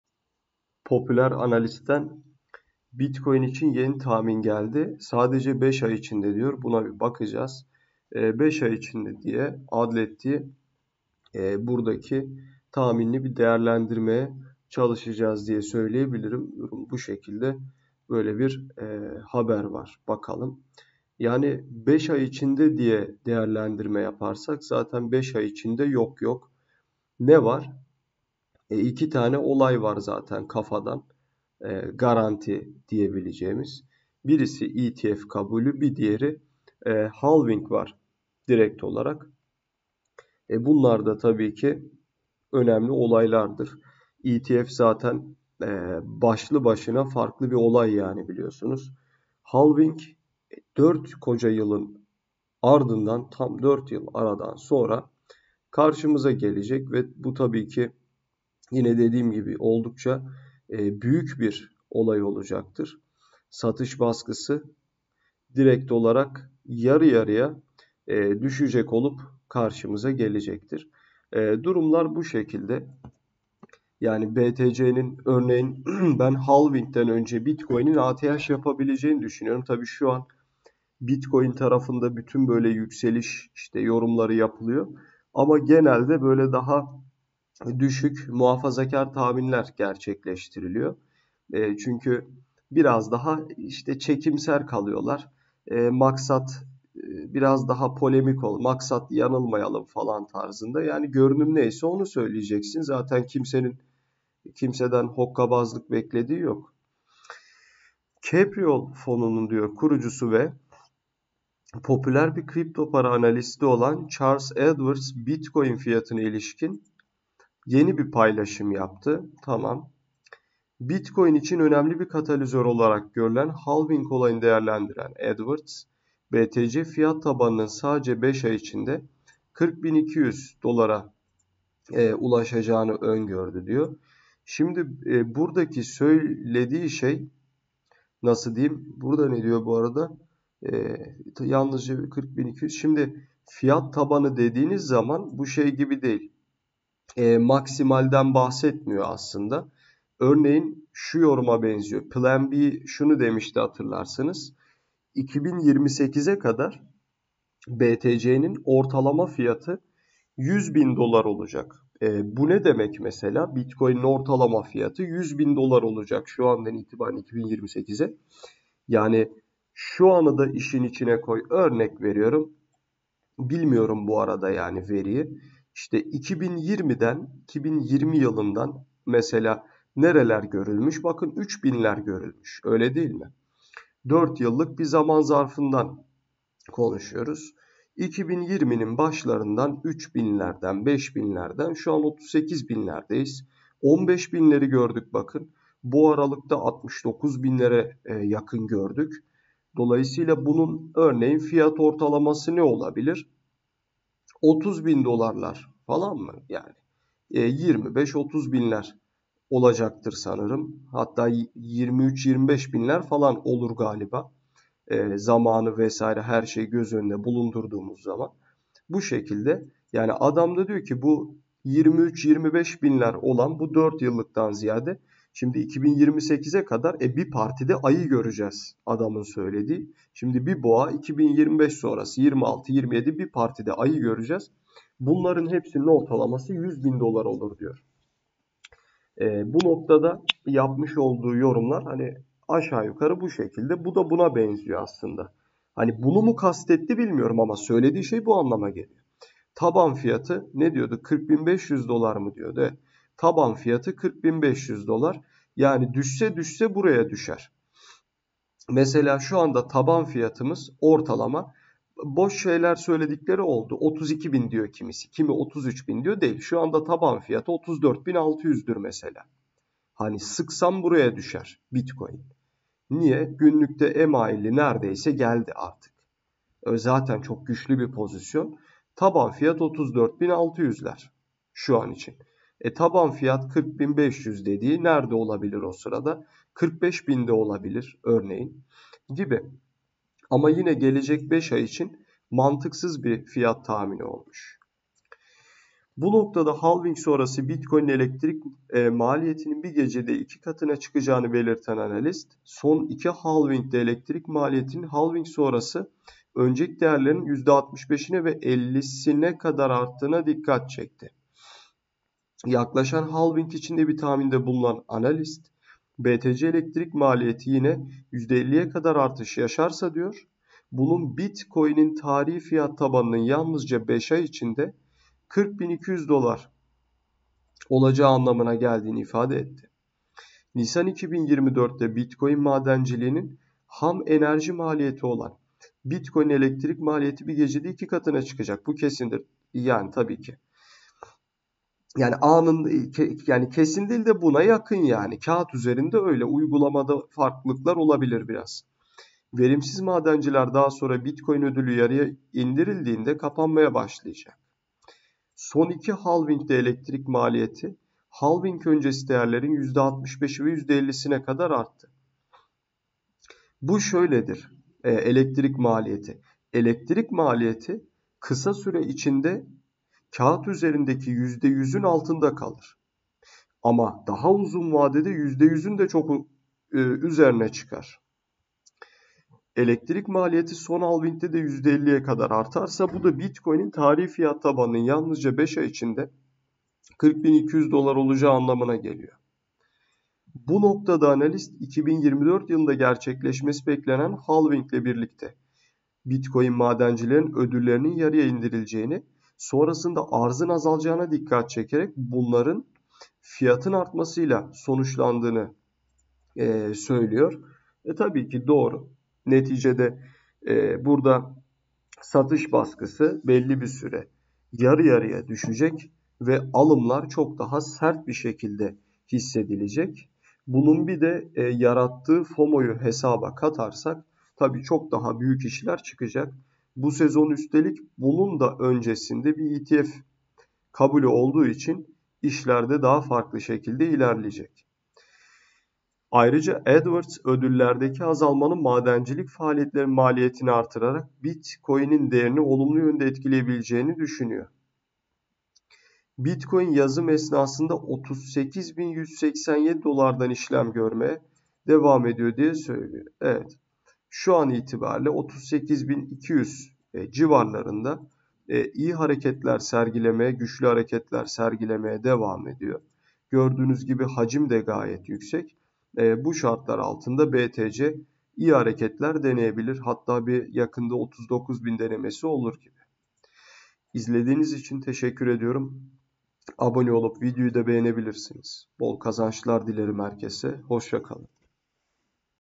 SON FIRSAT!🚀5 AY İÇİNDE...🔥ÜNLÜ UZMAN:"BİTCOİN'İN, DİP FİYATI 100.000$ OLACAK!"🚀ŞİMDİ ALDIN ALDIN..🔥
Bu videomuzda Bitcoin'in analizini gerçekleştirdik. Kısa ve uzun vadede neler yapabileceğini değerlendirdik, herkese iyi seyirler. Bitcoin an itibariyle 39000 pozisyonlarında ve destek tutunmaya çalışıyor. Alt kanala atması ile 37 bin seviyelerine kadar düşebilir ve ardından 30k kadar takip eder. Bitcoin'in yükselen trendi an itibairyle çalıştı fakat gözler 12 nisan 15.30'da açıklanacak ABD enflasyon oranlarında. Enflasyonun yüksek açıklanması durumunda Btc ve piyasalarda sert bir düşüş gelebilir. Önümüzdeki günlerde açıklanacak bilançolar da kaderimizi belirleyecek. Bitcoin kahini olarak bilinen peter brandt'dan yeni bir tahmin geldi. Son gelişmelerle birlikte bitcoin tarafında ciddi bir düşüş var ve fed faiz kararını açıkladı. Son gelişmelerle bitcoin'de haftalık kapanış geliyor. Bu videoda anlatılanlar yatırım tavsiyesi değildir.